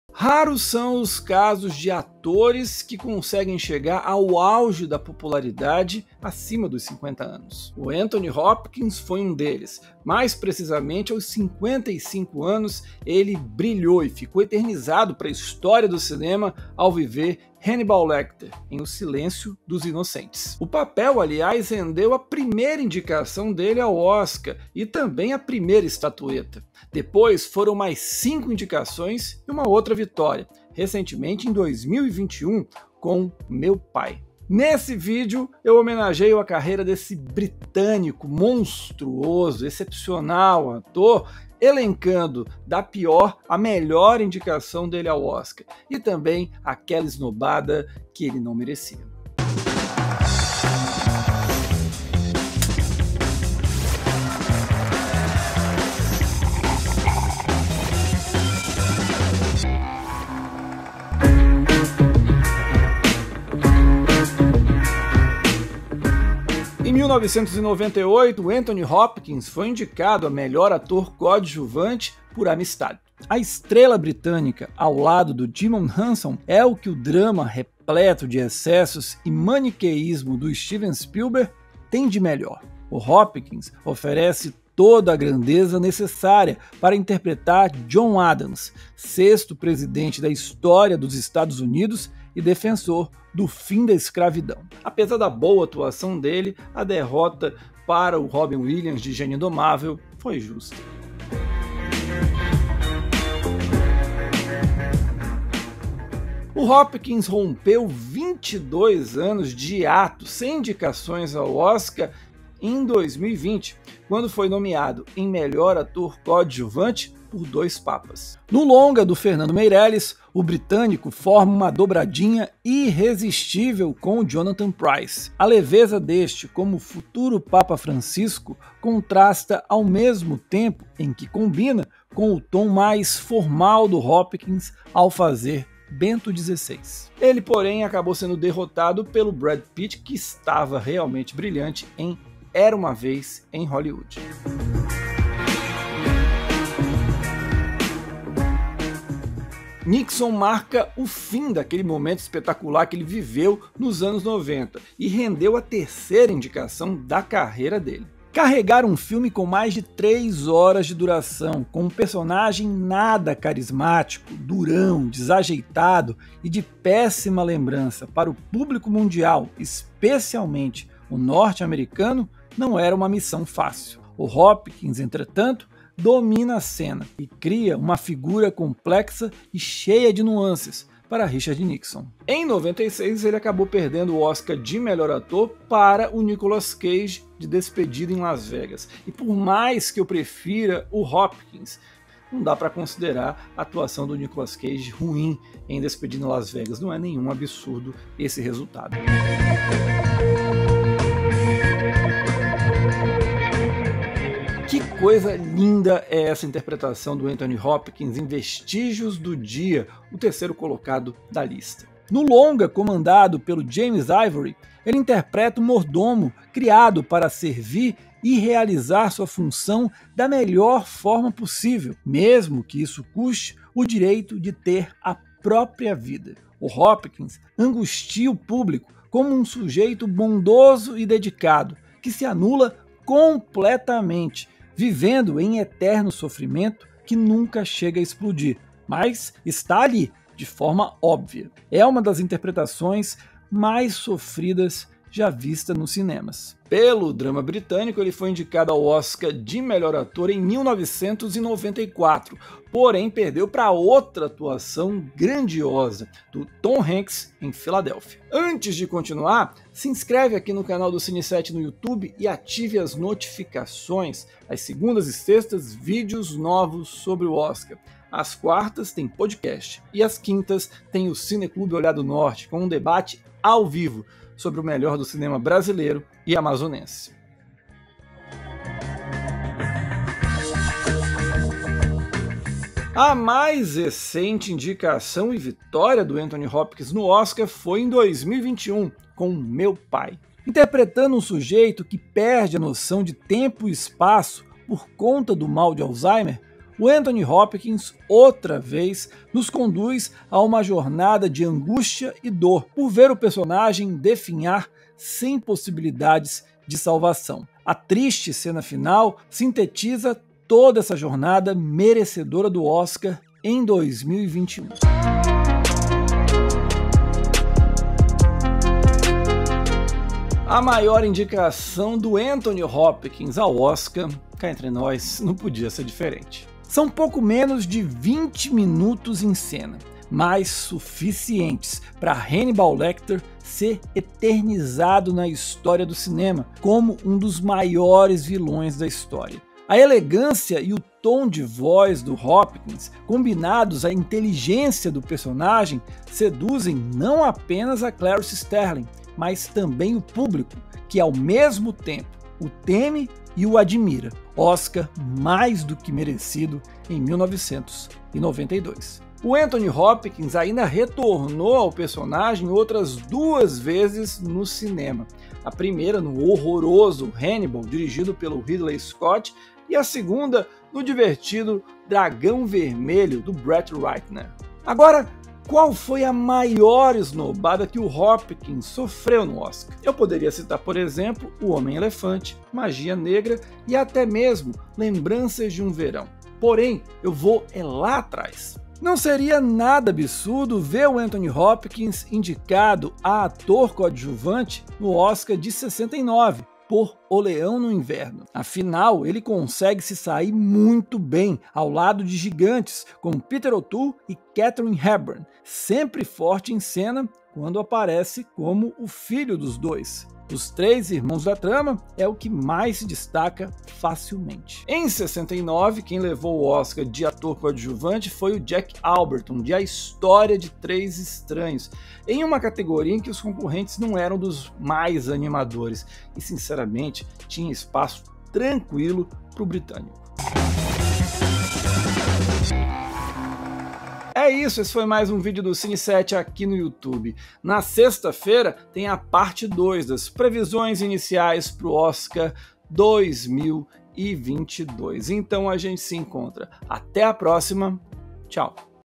Oscar: O Melhor e o Pior das Indicações de Anthony Hopkins
De "O Silêncio dos Inocentes" a "Meu Pai", Caio Pimenta analisa quais as melhores e piores indicações de Anthony Hopkins no Oscar.